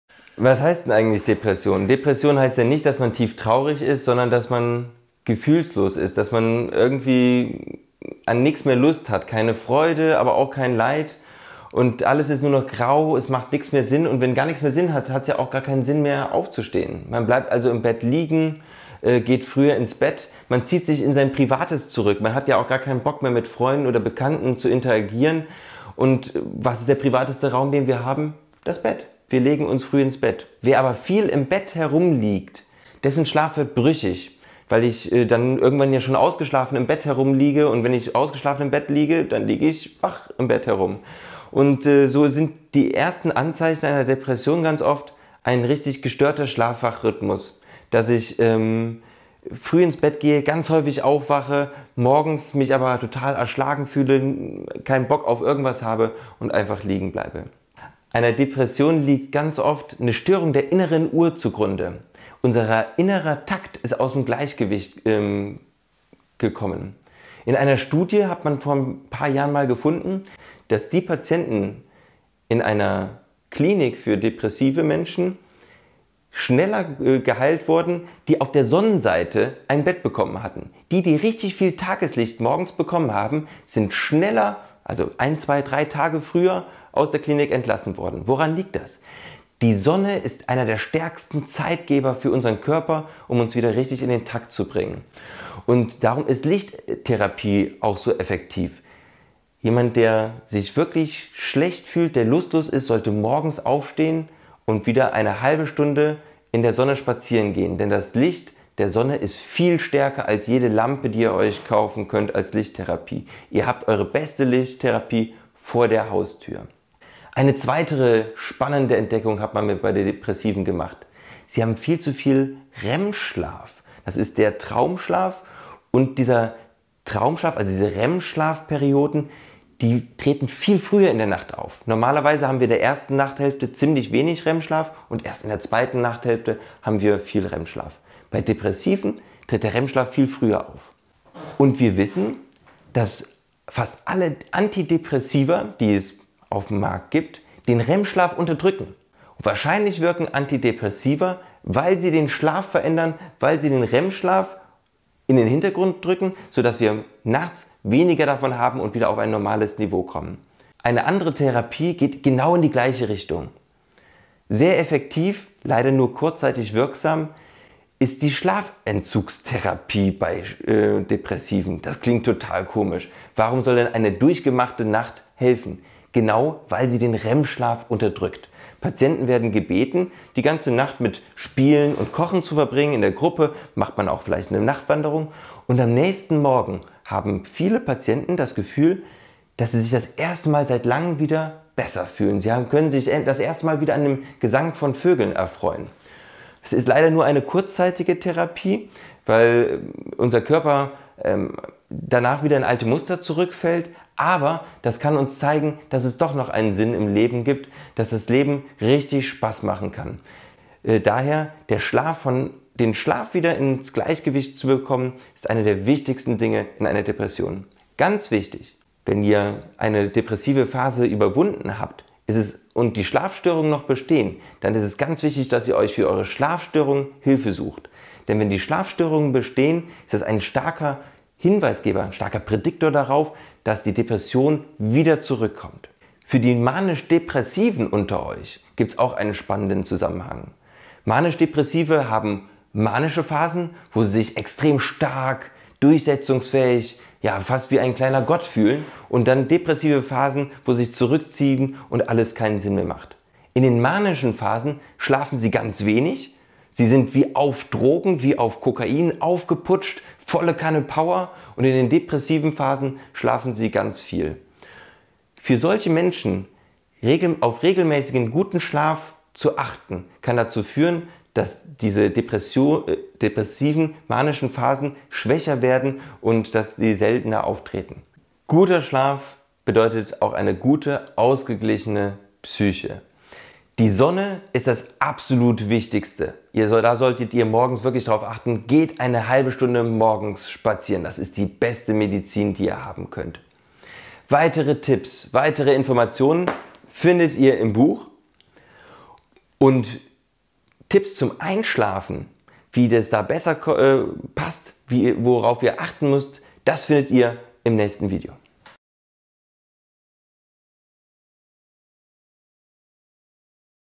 Ich komme nicht aus dem Bett – Depression?
BUCH ►
Kindle Version
Hörbuch (download)
Hörbuch (4 CDs)

In meinem Video beschreibe ich wie Depressionen mit der Veränderung des Schlafs zusammenhängen. Gehst du auch früh ins Bett, liegst trotzdem lange wach, kannst nicht schlafen und fühlst dich am Morgen erschlagen, müde und träge, ohne Lust und Kraft aufzustehen? In diesem Video erkläre ich was dahinter stecken könnte.

Patienten die in Zimmern mit mehr Sonnenlicht liegen werden schneller gesund. Die Sonne ist viel stärker als alle Lampen zur Lichttherapie und sie ist kostenlos. Die Sonne bringt eure innere Uhr wieder ins Gleichgewicht und damit auch das Gleichgewicht der Neurotransmitter in eurem Gehirn.
Eine Depression bewirkt einen Anstieg des REM-Schlafes. Interessanterweise wirken viele Antidepressiva, weil sie den REM-Schlaf unterdrücken. Genauso wie auch Schlafentzug als Kurzzeittherapie bei Depressionen eingesetzt wird.

Am Ende des Videos erkläre ich warum die Behandlung einer Schlafstörung bei depressiven Erkrankungen so wichtig ist und wie manisch-depressive Patienten mit einem geregelten Schlaf die Zahl der manischen und depressiven Episoden mindern können.